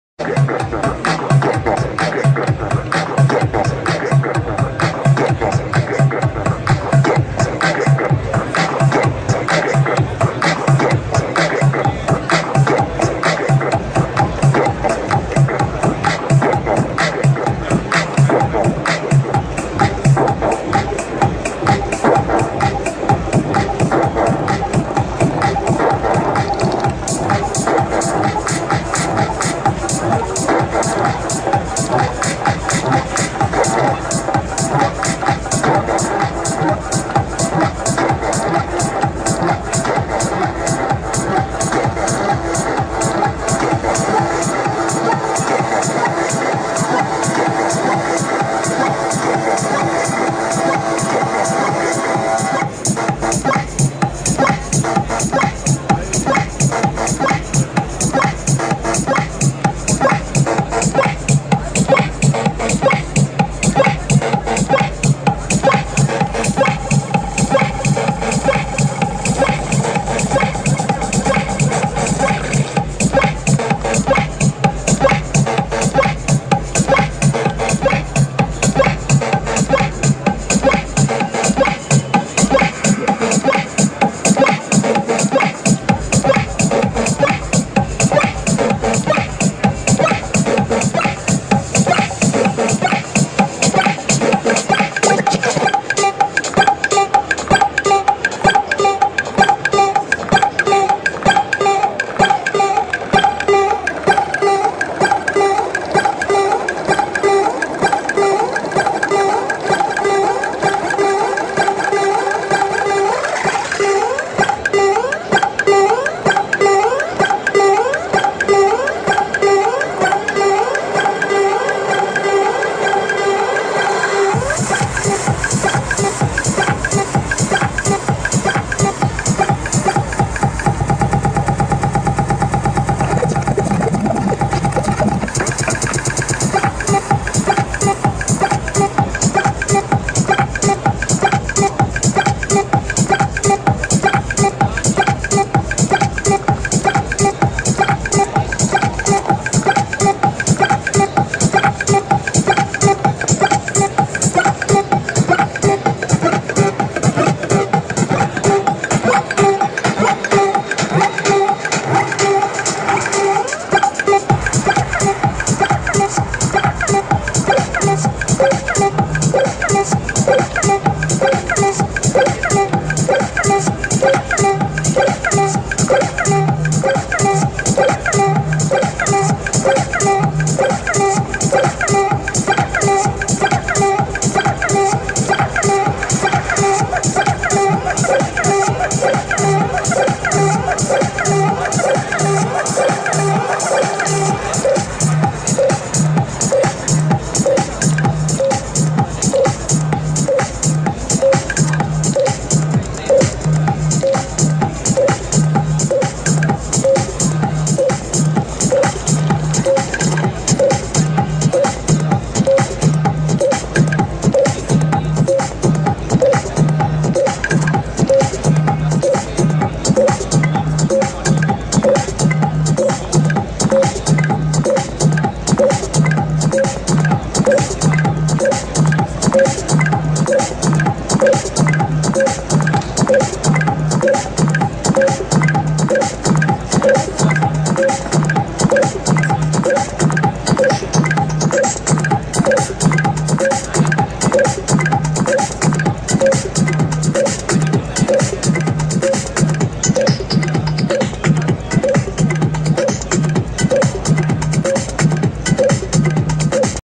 DJ TECH IMIX RELOAD MKII
i-Mix Reload MKII contains everything you need to plug into any club's house PA and perform.
This high-quality, lightweight and rugged DJ device is not only a hardware controller, for performing with included native instruments Traktor 3 LE and Deckadance LE software, but it also houses a professional digital-audio interface to feed your headphones and the sound system.
Built with serious: an extremely clean, high-quality soundcard feeds pristine audio signal to two stereo outputs and your headphones.
i-Mix Reload MKII has high-quality controls and extremely resistant ABS housing. It'll stay in place even when your fast-scratch technique gets aggressive. Carry less and do more with i-Mix Reload MKII.